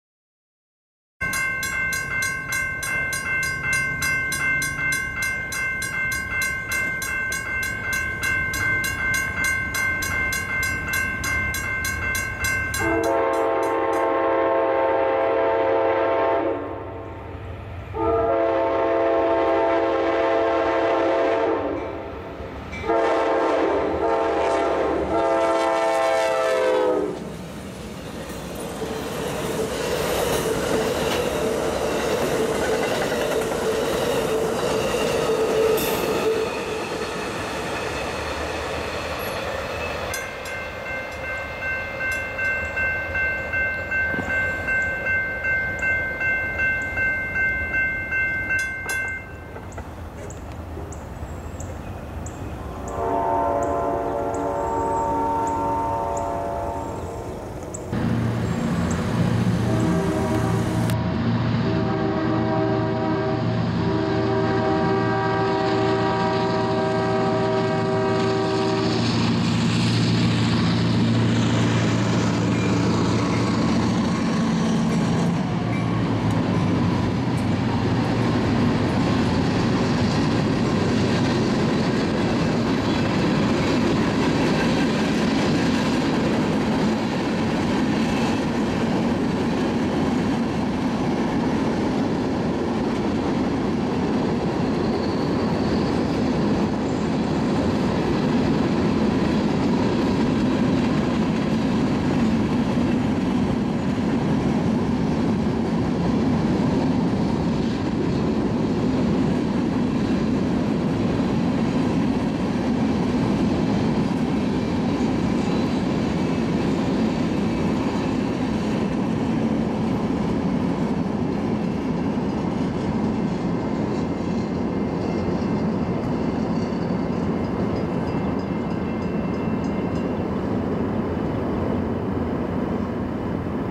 AMTK 76/194 & UP 805/601 Local in Cliff Cave County Park(March 9, 2015)
Yesterday, March 9, I arose early and headed to nearby Cliff Cave County Park to catch the northbound Amtrak Texas Eagle passing through the park.  About 20 minutes after I arrived, the train blasted through, led by AMTK 76 & AMTK 194, with a 1700 Series heritage baggage car in tow in front of the 7 Superliner cars.  About an hour later, the daily UP local came blasting by, led by UP 805 & UP 601.  Shortly after the local passed, I hurried home to a hearty breakfast!!!  Enjoy!!!